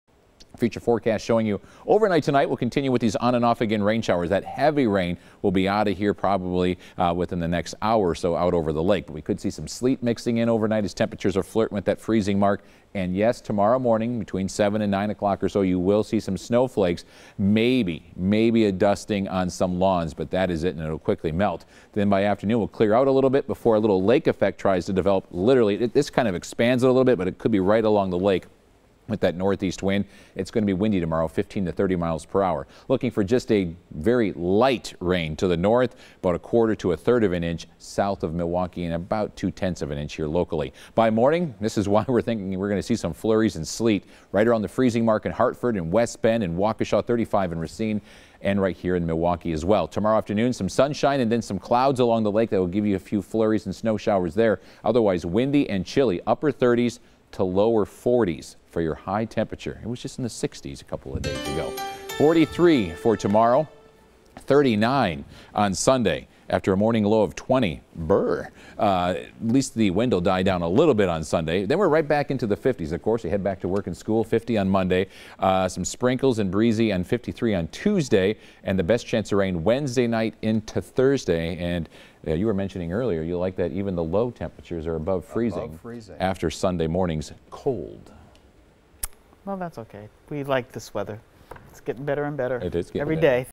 Possible dusting of snow Saturday morning
Before the storm leaves the area Saturday morning, areas south of Milwaukee could see a few snowflakes. Areas well north of Milwaukee might not see a drop of rain.